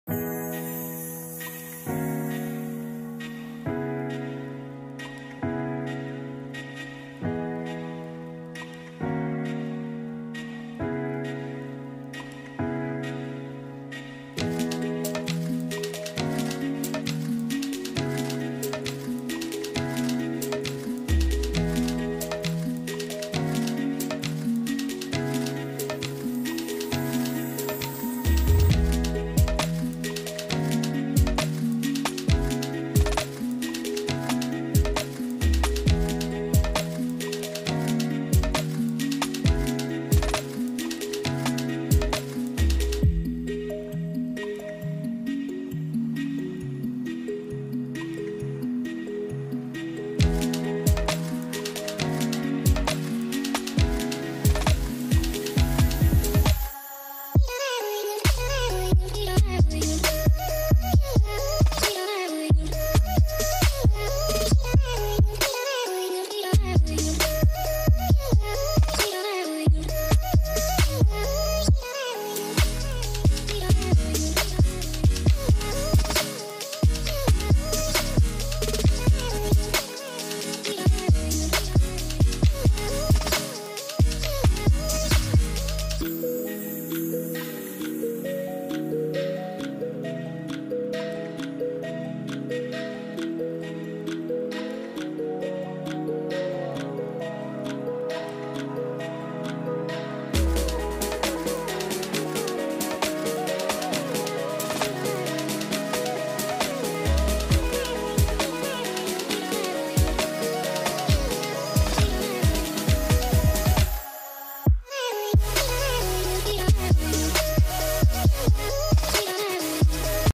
Draw a evening scenery with step by step, oil pastal drawing.
easy evening scenery with oil pastal colors.

useful products,
page
oil pastal colors (Doms)
pencil